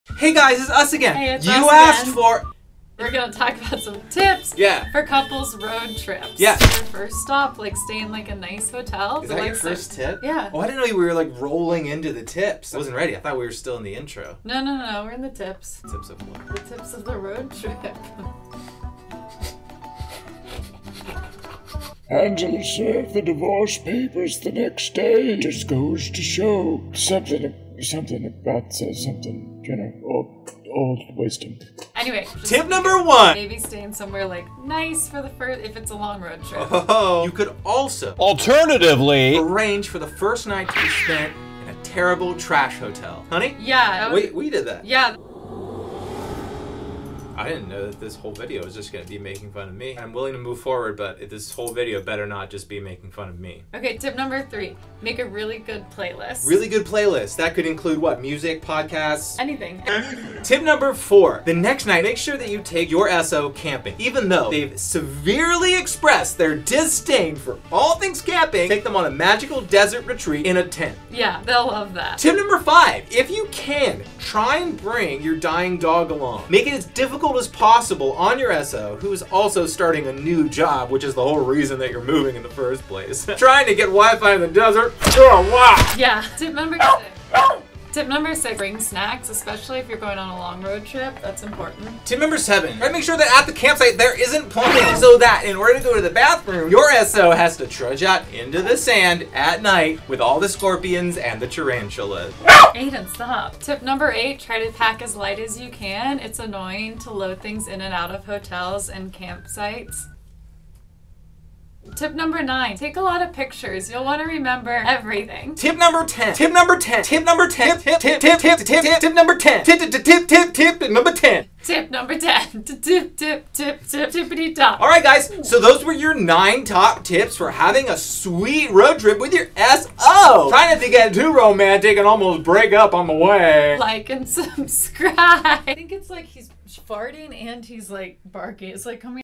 Road Trip Tips for Couples!!1 | Married Comedy
get in the car and kill the planet, happy couples! Just a few casualroad trip tips for couples and other people who get in cars for long periods and get out in a new place!